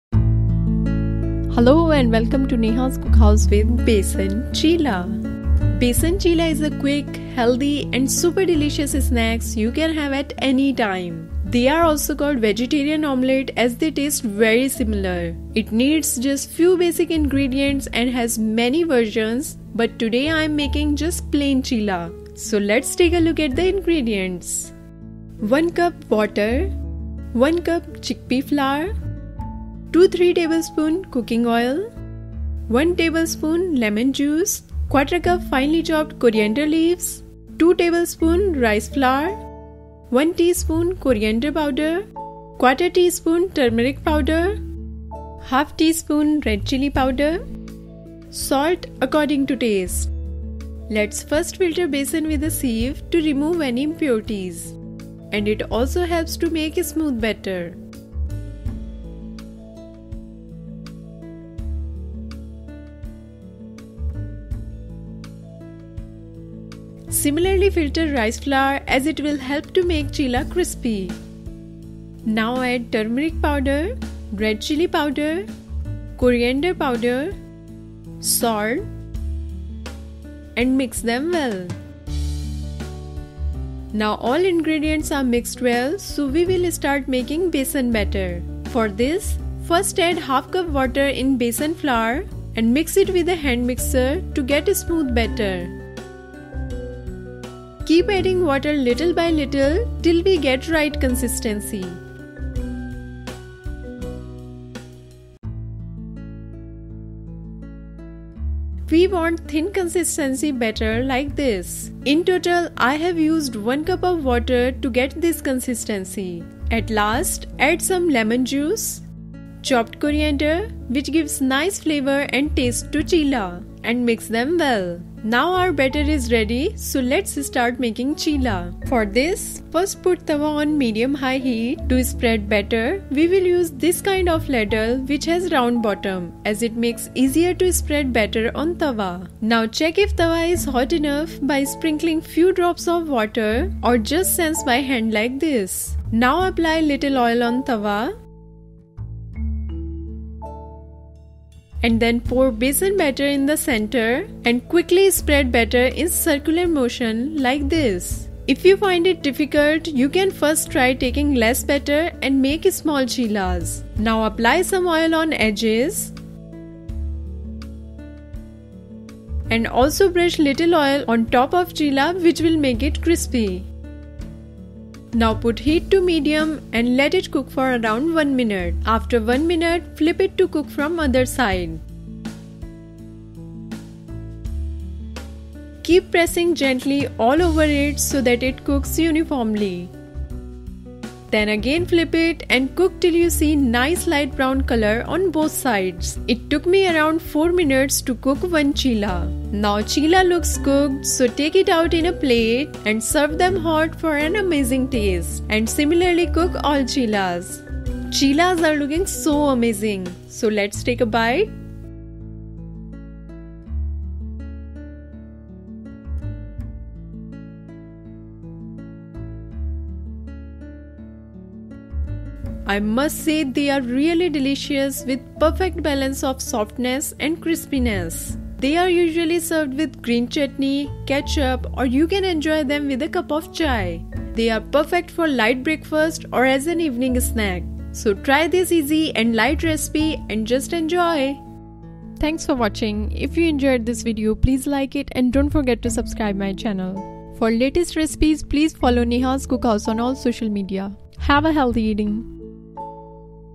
Besan Cheela Recipe | Vegan Omelette Recipe | How to Make Besan Cheela | Nehas Cookhouse
Besan Cheela is a quick, healthy and super delicious snacks you can have at any time. They are also called vegetarian omelets as they taste very similar. It needs just few basic ingredients and has many versions but today I am making just plain cheela. They are usually served with green chutney, ketchup or you can enjoy them with a cup of chai. They are perfect for a light breakfast or as an evening snack. Try this easy and light recipe and just enjoy.

Other Names:

Vegan Omelete, Vegetarian Omelete, Vegan Omelette, Vegetarian Omelette, Besan Ka Chilla, Besan Ka Cheela

Ingredients:

1 cup Water
1 cup Chickpeas Flour
2-3 tbsp Cooking Oil
1 tbsp Lemon Juice
¼ cup Fresh Coriander Leaves (finely chopped)
2 tbsp Rice Flour
1 tsp Coriander Powder
¼ tsp Turmeric Powder
¼ tsp Red Chilli Powder

Instructions:

1. Lets first filter besan with a sieve to remove any impurities and it also helps to make a smooth batter. Similarly, filter rice flour as it will help to make cheela’s crispy. Now add turmeric powder, red chilli powder, coriander powder, salt and mix them well.

2. Now all ingredients are mixed well so we will start making besan batter. For this, first add ½ cup water in besan flour and mix it with a hand mixer to get smooth batter. Keep adding water little by little till we get the right consistency. We want thin consistency batter like this, in total I have used 1 cup water to get this consistency.

3. At last add some lemon juice, chopped coriander which gives nice flavor and taste to cheela and mix them well. Now our batter is ready so lets start making cheela.

4. For this first put a tawa on medium high heat. To spread batter we will use a ladle which has round bottom as it makes easier to spread batter on tawa. Now check if tawa is hot enough by sprinkling few drops of water or just sense by hand like this. Now apply little oil on tawa and then pour besan batter in the center and quickly spread batter in circular motion like this. If you find it difficult you can first try taking less batter and make small cheelas.

5. Now apply some oil on edges and also brush little oil on top of cheela which will make it crispy. Now put heat to medium and let it cook for around 1 min. After 1 min flip it to cook from the other side, keep pressing gently all over it so that it cooks uniformly. Then again flip it and cook till you see nice light brown color on both sides. It took me around 4 mins to cook one cheela.

6. Now cheela looks cooked so take it out in a plate and serve them hot for an amazing taste. Similarly cook all cheela’s.

Tips & Tricks:

1. You can also add curd instead of lemon juice.
2. To make it more versatile you can add any herb or finely chopped veggies of your choice.
3. If you want to make it extra special you can also fill it with stuffing of your choice.